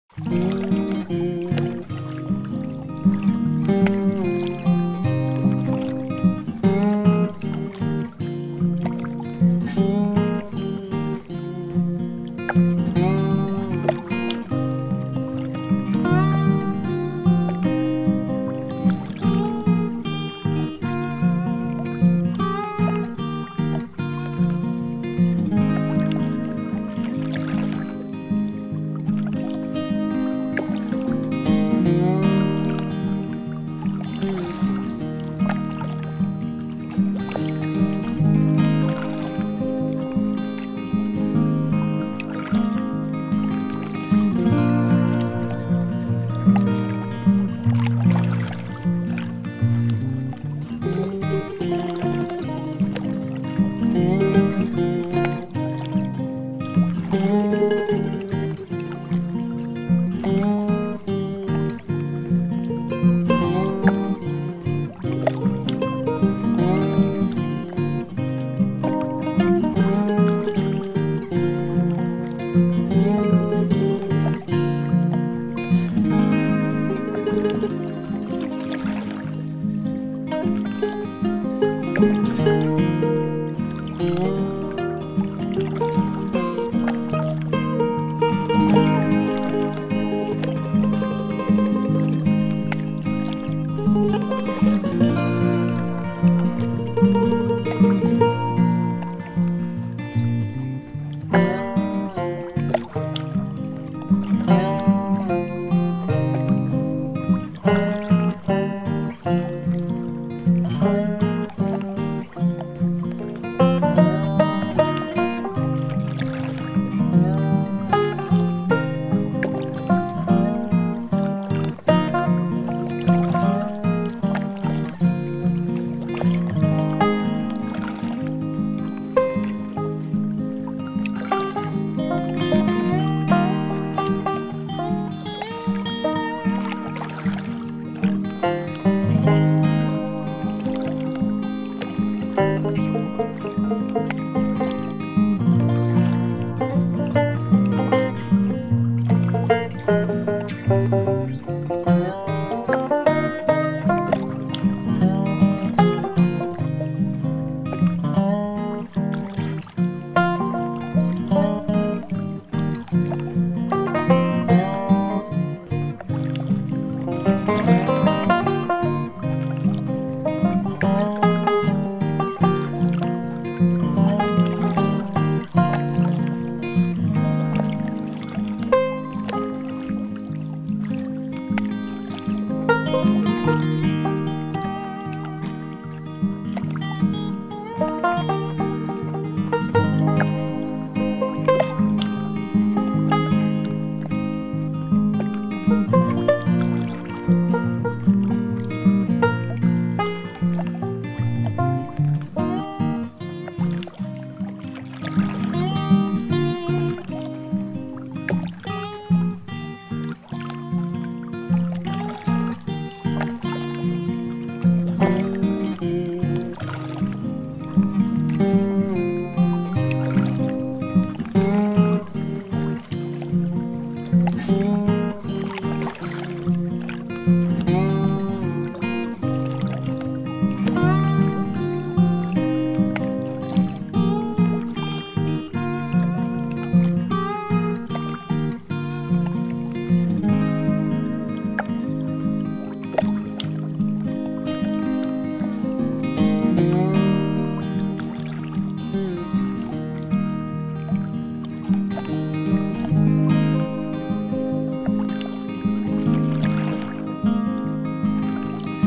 VR 360 Date with Lana. Your new girlfriend
Meet your new girlfriend Lana. She is young, beautiful and very sexy, you will definitely fall in love with her. What about sitting together on the lake shore and enjoy relaxing in nature in the new episode of our girlfriend simulator? This is a VR 360 video, get more positive emotions watching in your virtual reality mask